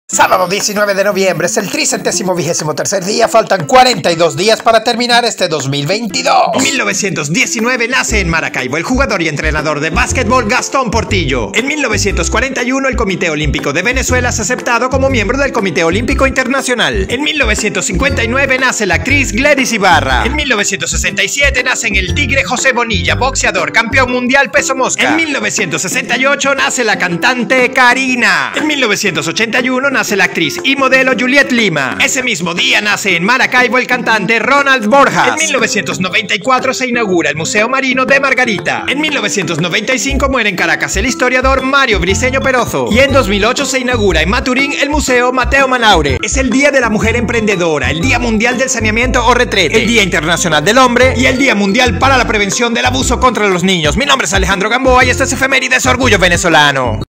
CURADAS - Efemérides Orgullo Venezolano | Sábado 19 de noviembre de 2022
19 de noviembre 2022, sábado
Es el tricentésimo vigésimo tercer día, faltan 42 días para terminar este 2022.
1813: Alexander Betts cartógrafo alemán que junto a Agustín Codazzi traza el plano de la Colonia Tovar.
1919: nace en Maracaibo Gastón Portillo jugador y entrenador de basquetbol; exaltado al Salón de la Fama del Deporte Venezolano en 1992. (f. 1994).
1941: el Comité Olímpico de Venezuela es aceptado como miembro del Comité Olímpico Internacional.
1943: nace el beisbolista cubano- venezolano Aurelio Monteagudo "Monty" (f. 1990).
1959: nace en Caracas la actriz Gledys Ibarra.
1967: nace en El Tigre, estado Anzoátegui, el boxeador José Bonilla Campeón Mundial Peso Mosca (f. 2002).
1968: nace en La Guaira el beisbolista Luis Raven.
1968: nace la cantante peruano-venezolana "Karina".
1981: nace la actriz y modelo Juliet Lima.
1981: nace en Maracaibo el cantante Ronald Borjas.
1994: se inaugura el Museo Marino de Margarita.
1995: muere en Caracas el historiador Mario Briceño Perozo (n. 1917).
2008: se inaugura el museo Mateo Manaure en Maturín.
Es el Día Internacional de la Mujer Emprendedora.
Proclamado por la Organización de las Naciones Unidas desde el 2013 es el Día Mundial del saneamiento o Retrete.
Desde 1992 es el Día Internacional del Hombre.
Es el Día Mundial para la prevención del abuso contra los niños.
Podcast diario Nro 750, Año 3